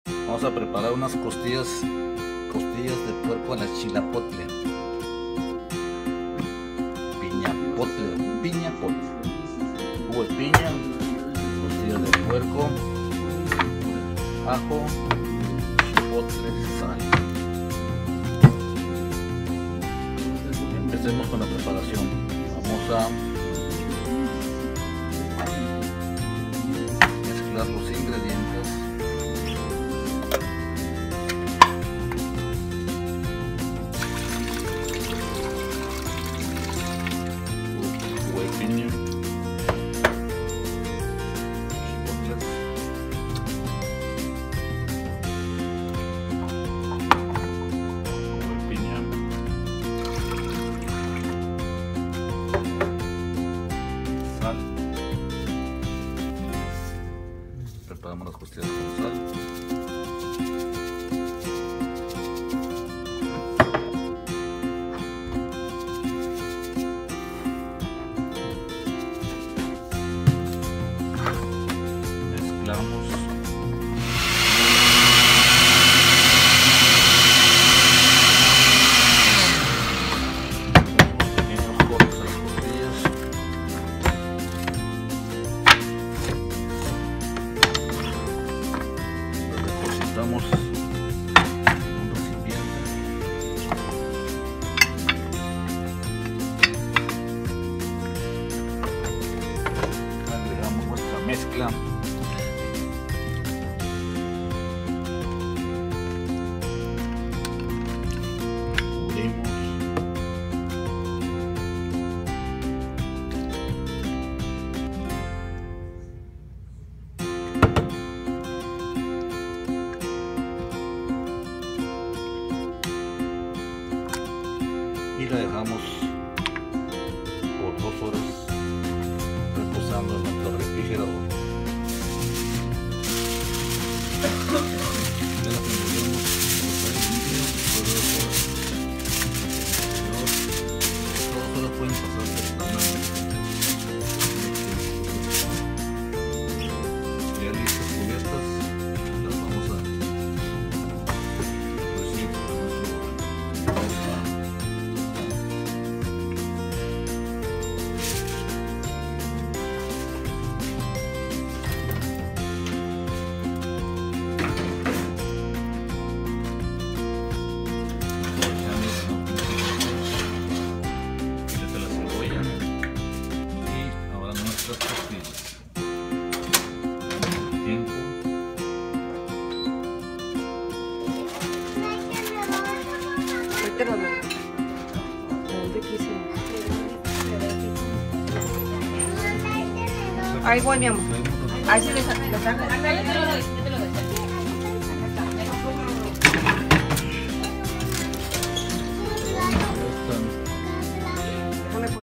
Costillas Gourmet de Cerdo al Horno
Hoy probamos estas deliciosas costillas de cerdo al horno, muy ricas, se las recomendamos